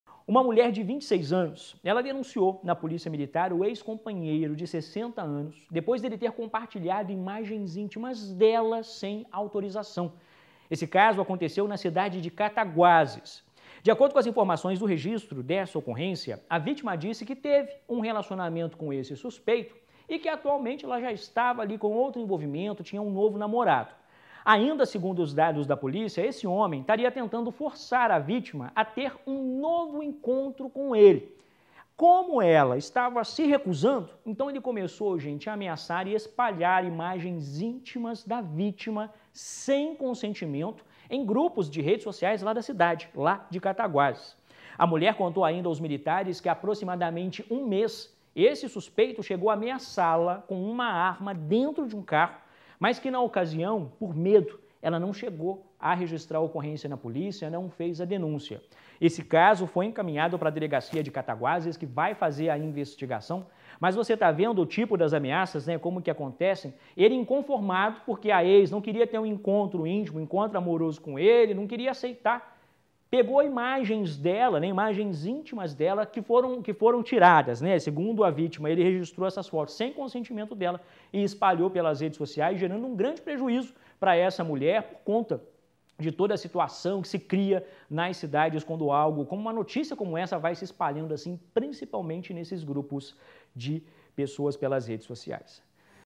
Mulher denuncia ex por compartilhar imagens íntimas
Uma mulher de 26 anos denunciou na polícia militar o ex-companheiro, de 60 anos, depois dele ter compartilhado imagens intimas dela, sem autorização. De acordo com as informações do registro dessa ocorrência, a vítima disse que teve um relacionamento com o suspeito e que atualmente tinha um outro namorado.